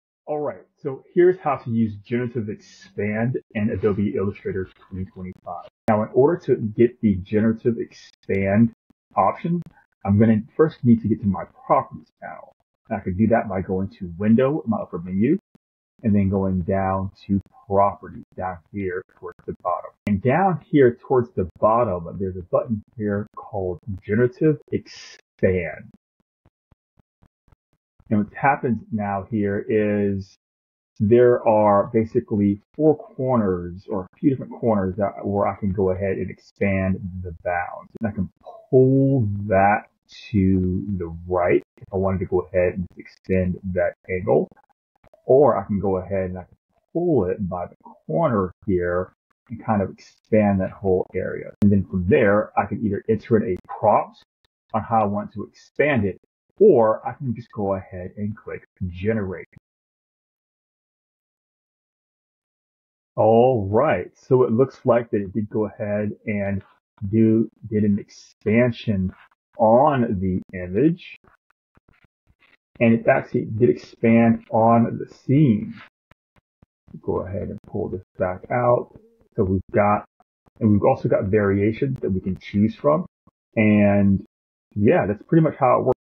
Adobe Illustrator: Generative Expand Tutorial
Adobe Illustrator 2025 just handed designers a new superpower with Generative Expand
↳Take an image
↳Grab a corner
↳Pull
Watch the scene grow beyond its original frame
You can also guide it with prompts to create exactly what you imagine
Skyline? Forest?
A few more characters in the shot? Done
It feels like stepping through the edges of your canvas into a bigger world you didn’t know was there
↳For brand designers, that means more flexibility in layouts
↳For illustrators, richer storytelling without starting from scratch
↳For marketers, visuals that adapt to every format instantly
The possibilities aren’t just bigger
They’re endless
What would you expand first?
— Kendol Mason
Graphic Designer